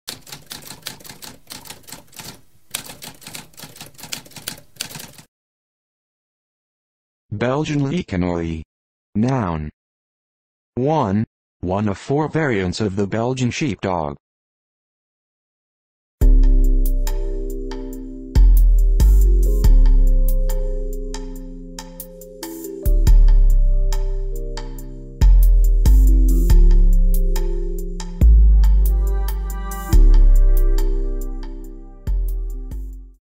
What does Belgian Laekenois mean?
A spoken definition of Belgian Laekenois.

Intro Sound:
Typewriter - Tamskp